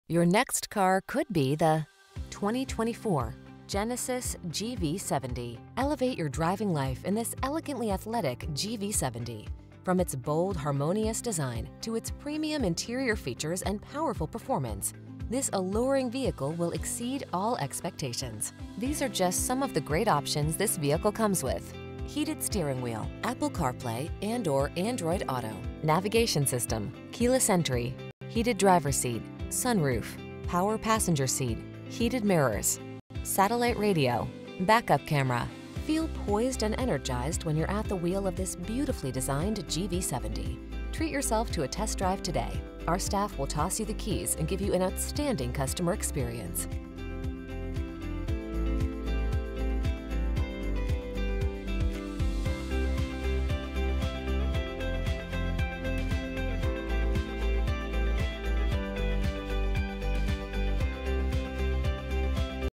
2024 Genesis GV70 at Oxmoor Hyundai Louisville & Lexington, KY G428
Savile Silver New 2024 Genesis GV70 2.5T available in Louisville, Kentucky at Oxmoor Hyundai. Servicing the Lexington, Elizabethtown, KY, New Albany, IN, Jeffersonville, IN area.

Oxmoor Hyundai
8107 Shelbyville Rd

2024 Genesis GV70 2.5T 22/28 City/Highway MPG